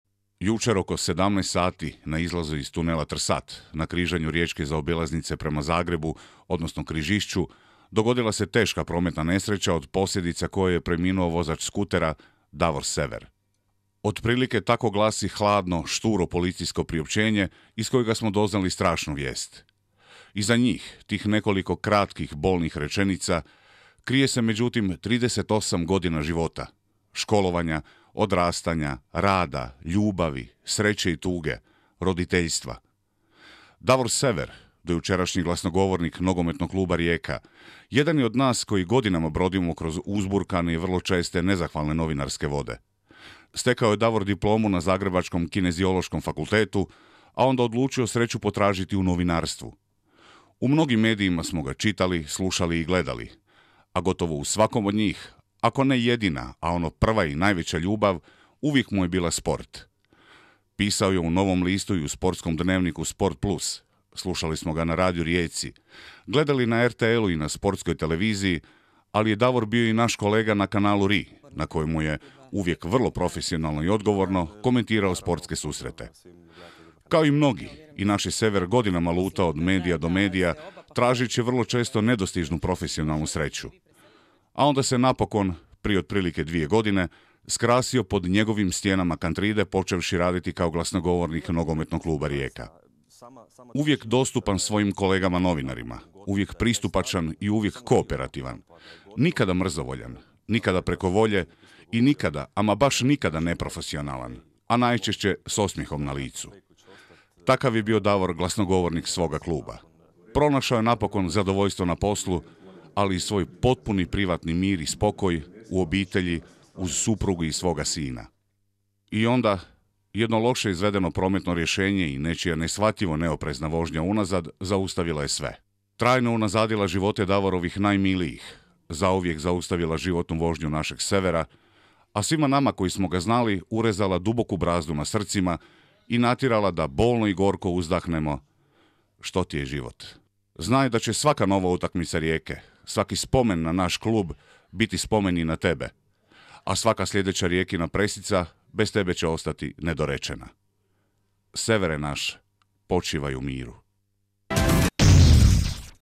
IN MEMORIAM DAVOR SEVER
Prijatelji, znanci, kolege, navijači "Rijeke" i, dakako, njegovi najmiliji danas u 15,00 sati na groblju Kozala oprostit će se od tragično preminulog Davora Severa, glasnogovornika HNK Rijeke i nekadašnjeg novinara.
Kanal Ri od svog se prijatelja i bivšeg kolege oprostio pričom o njegovom prekratkom životu.
Severe naš, volimo te! Počivaj u miru!